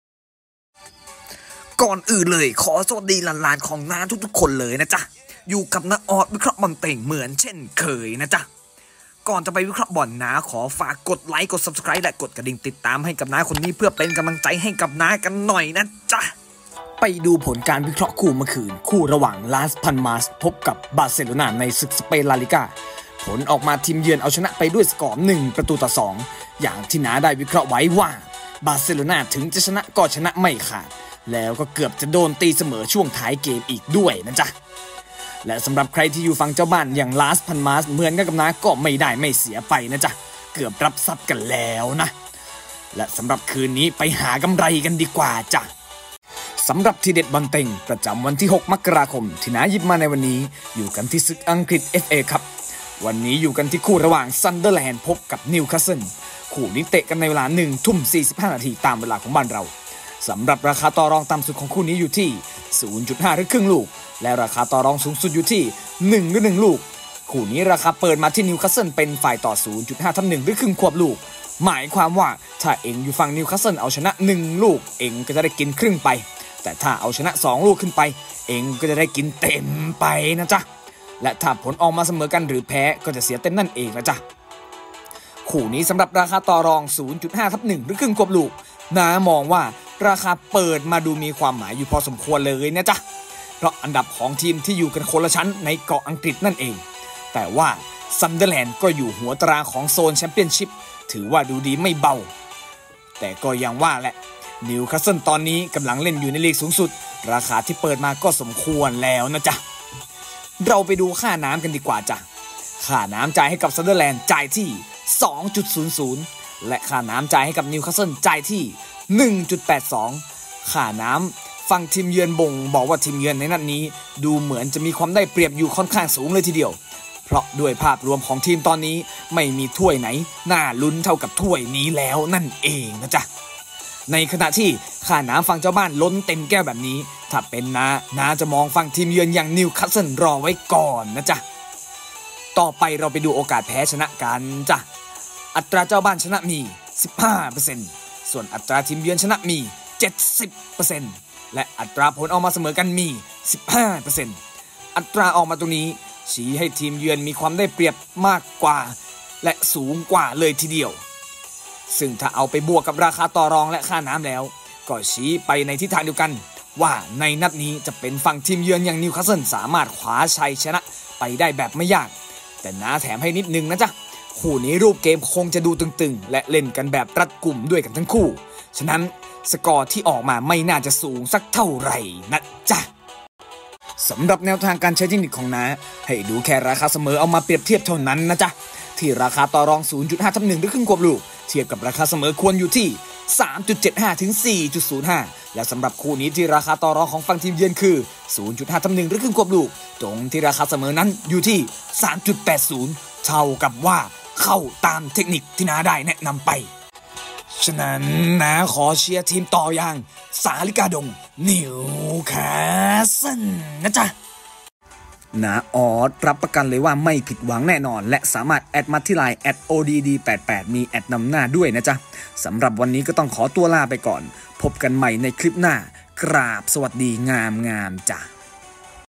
วิเคราะห์บอลวันนี้ บอลเต็ง บอลเดี่ยว ทีเด็ดบอล l 6 ม.ค. 67 l น้าอ๊อด หวานเจี๊ยบ l
วิเคราะห์บอลเต็ง วันที่ 6 มกราคม 2567 น้าอ๊อด จัดให้ หวานเจี๊ยบบบบ
ขอรับทีเด็ดฟรี ก่อนใคร แอดไลน์ ID @odd88
สนใจทีมอื่นๆ ก็สามารถ คอมเมนต์แนะนำกันได้เลย นะจ๊ะ
วิเคราะห์บอล วันเสาร์ ที่ 6 มกราคม 2567

00:00| INTRO
00:16| ผลการวิเคราะห์
00:55| ทีเด็ด Vip
01:06| ซันเดอร์แลนด์ - นิวคาสเซิ่ล
03:07| แนวทางการใช้เทคนิค
03:34| ฟันธง
03:41| สุดท้ายแอดไลน์ + กลุ่มนำเล่น VIP